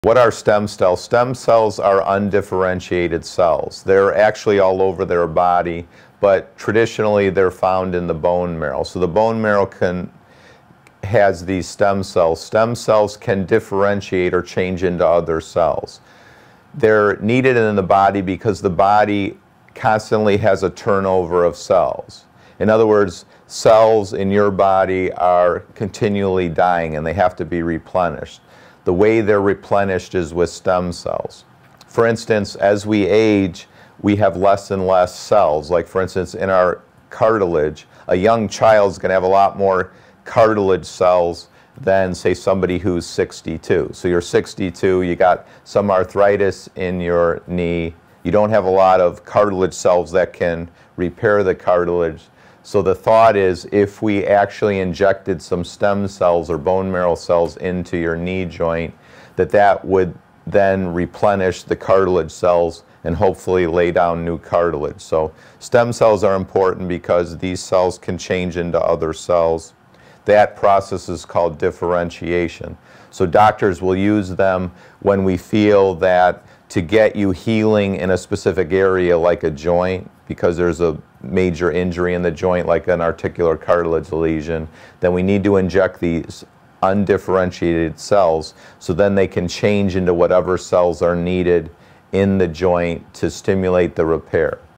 What are stem cells?
Stem cell therapy using Bone Marrow Prolotherapy injections is available as a same-day procedure at Caring Medical in Fort Myers, Florida, for people suffering from osteoarthritis and chronic pain. In this video, Medical Director Ross Hauser, MD explains what stem cells are, and the use of mesenchymal stem cells for cartilage repair and joint pain when used in the Prolotherapy solution. If you are interested in learning more about Stem Cell Prolotherapy as an alternative to surgery,

Contact our team to tell us more about your case and see if you are a good candidate for our treatments: